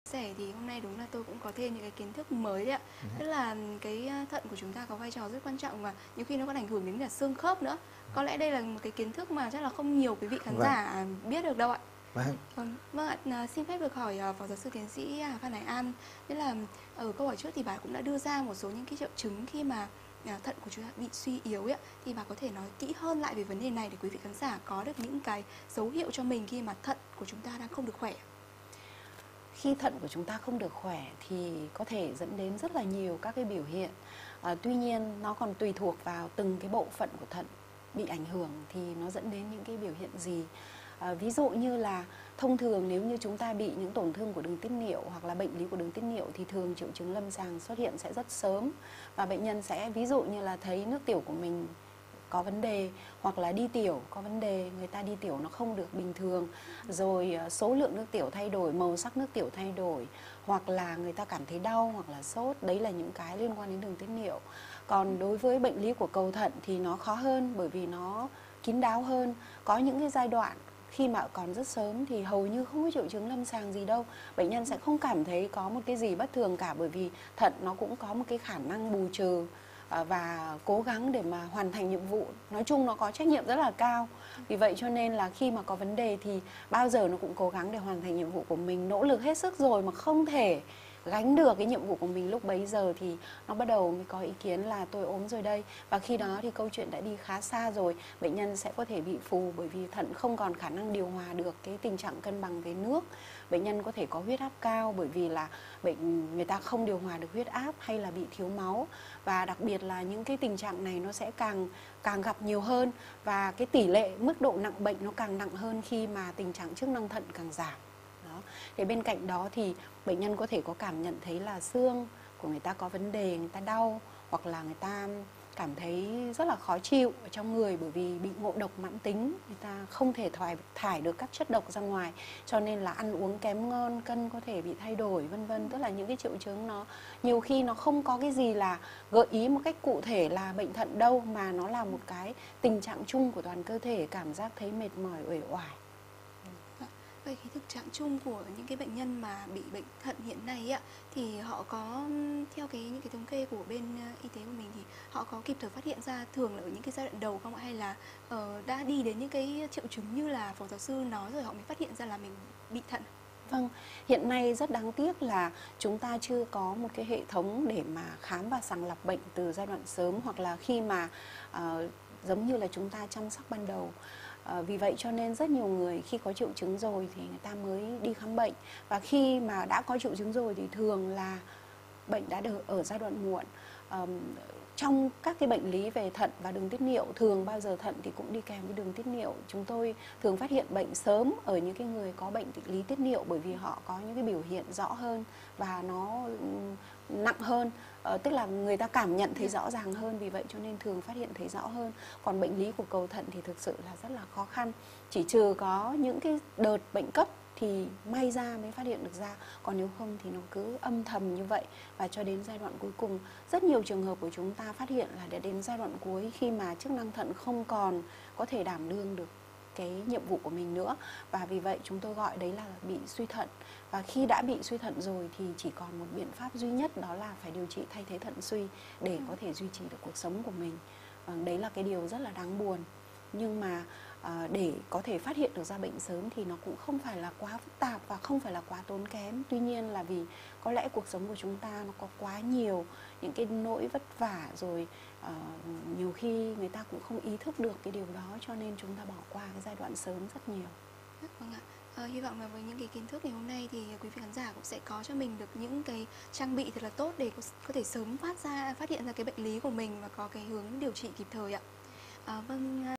Những biểu hiện khi thận suy yếu
🌺 Cùng nghe chia sẻ của chuyên gia:
🎐 PGS.TS. Hà Phan Hải An-Phó Chủ tich Hội Tiết niệu-Thân học Việt nam, Giảng viên cao cấp phân môn Thận học, Bộ môn Nội Tổng hợp, Trường Đại học Y Hà Nội, Nguyên Trưởng khoa Thận - Lọc máu, Bệnh viện Hữu nghị Việt Đức, Trưởng phòng Hợp tác Quốc tế, Đại học Y Hà Nội.
🎐 PGS.TS. Đậu Xuân Cảnh- Giám đốc Học viện Y Dược học cổ truyền Việt Nam.